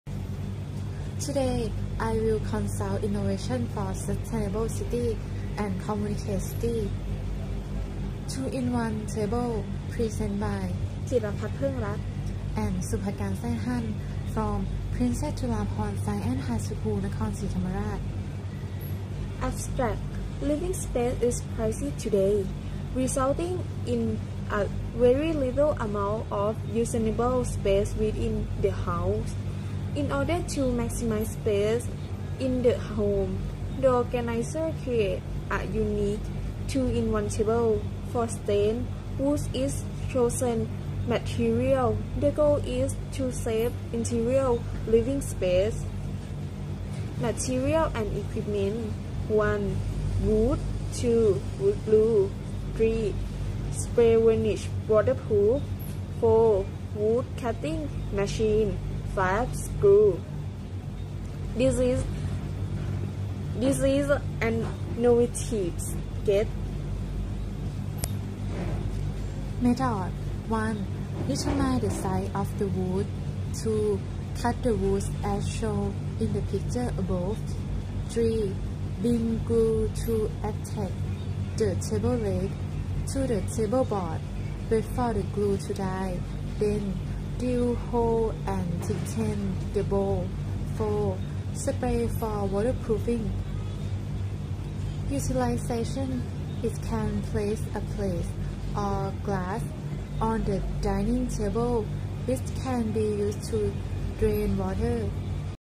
S183 2 in 1 Table | KLESF International Challenge 2022
[KLESF International Challenge 2022]

Number: S183
Title of Project: 2 in 1 Table
Name of participant #1: Jiraphat Poengrat
Name of participant #2: Supakan Saehun
Name of participant #3:
Name of School: Princess Chulabhorn Science High School  Nakhon Si Thammarat
Project theme: Sustainable Cities and Communities (SDG 11)
Category: Secondary school (13 - 18 years old).

Abstract of Project:
Living space is pricey today. resulting in a very little amount of useable space within the house. In order to maximize space in the home, the organizer created a unique 2-in-1 table. For strength, wood is the chosen material. The goal is to save interior living space.